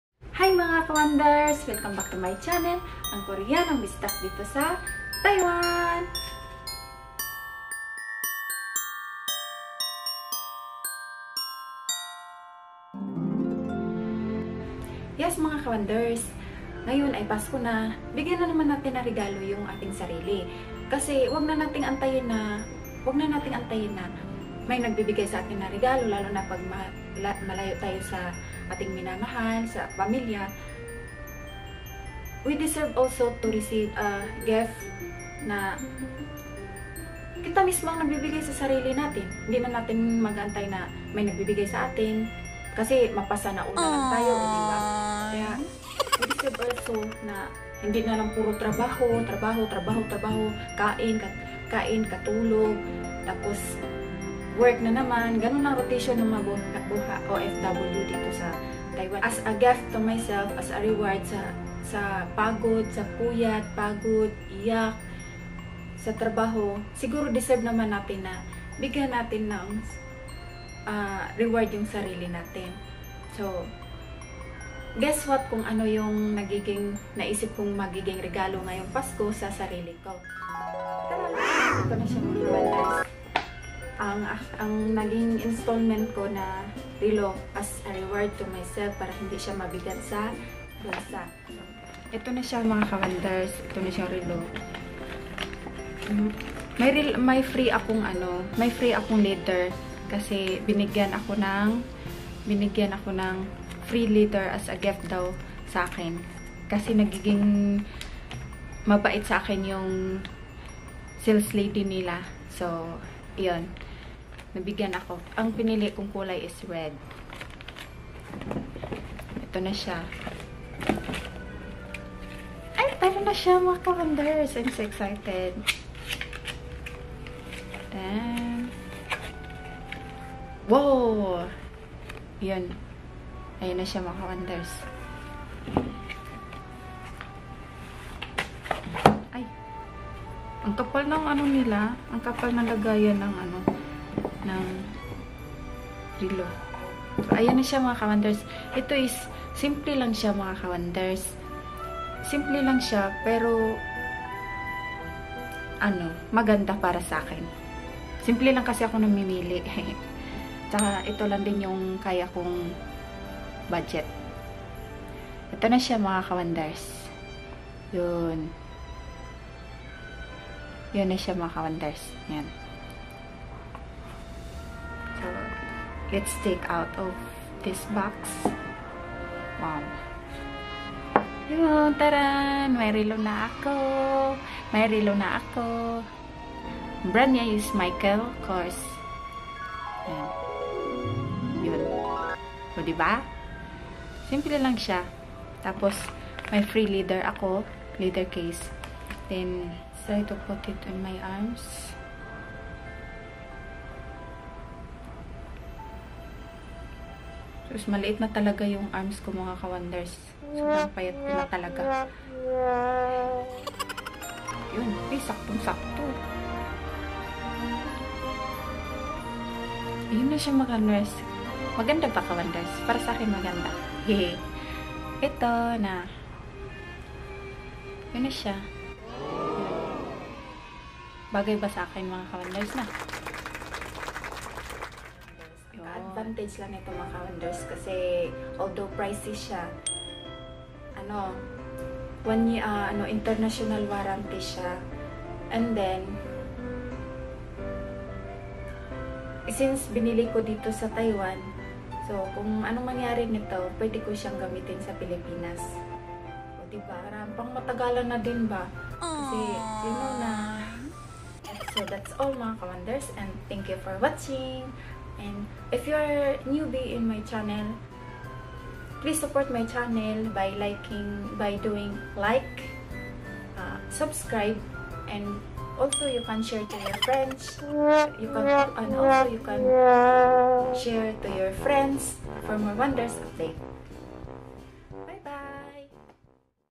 HOW TO REWARD YOURSELF ON CHRISTMAS | KOREANANG BISDAK | Investment for Yourself | SelfReward
MERRY CHRISTMAS KAWANDERS! This is how I gave reward to myself after all hardships as Overseas Filipino Worker OFW in this foreign land of Taiwan.